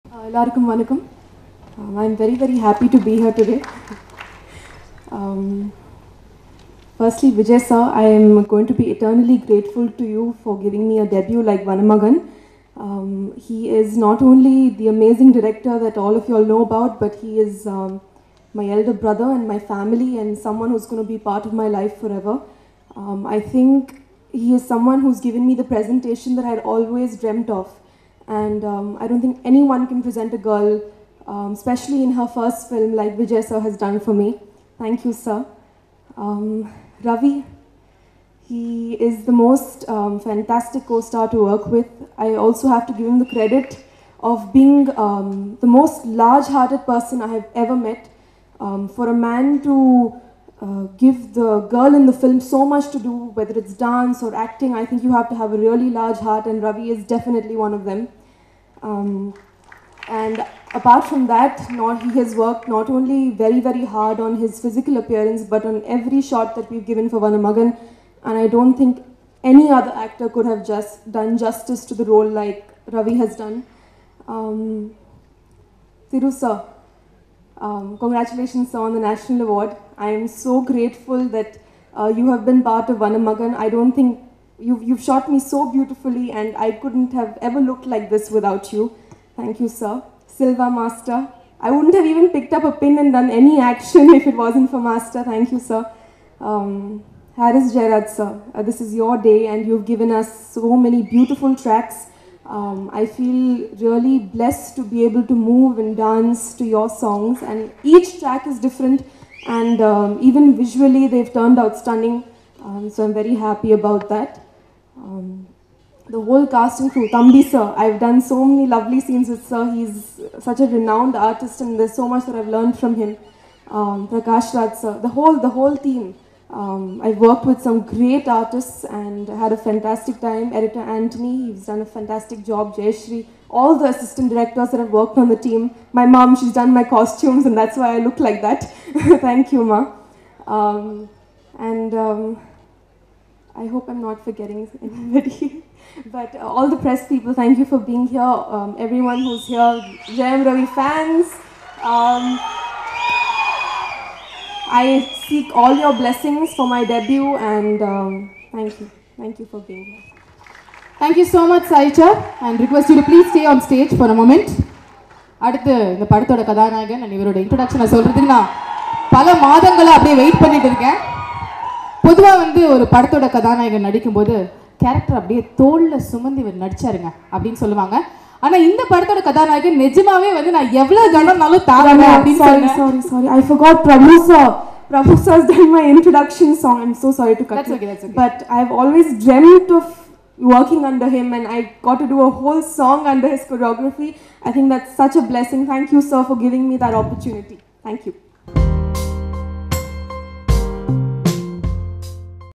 Vijay is like my elder brother - Sayesha Saigal ! | Vanamagan Audio Launch
விஜய் எனக்கு அண்ணன் மாதிரி - சாயீஷா!